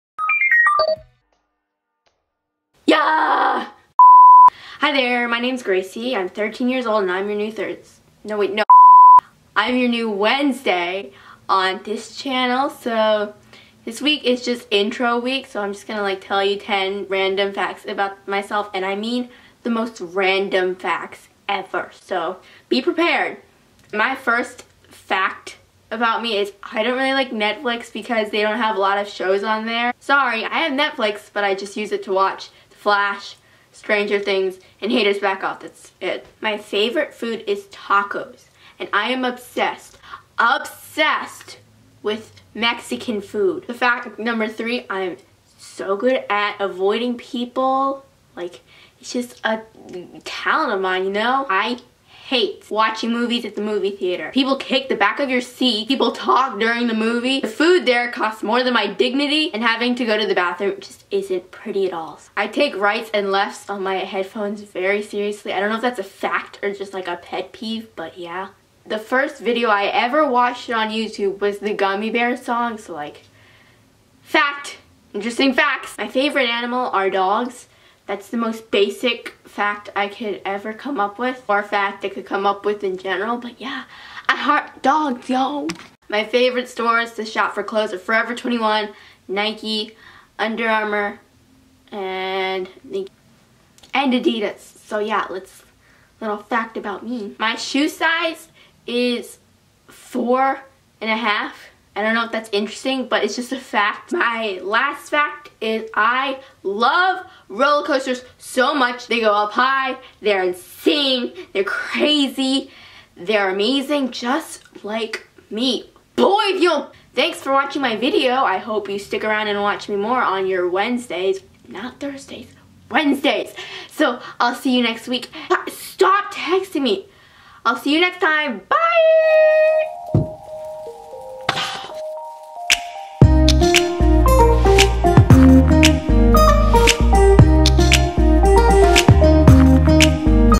HI.I'm Gracie
Hi! My names Gracie and I'm your new wednesday on this channel! I hope you stick around and keep watching, thanks :)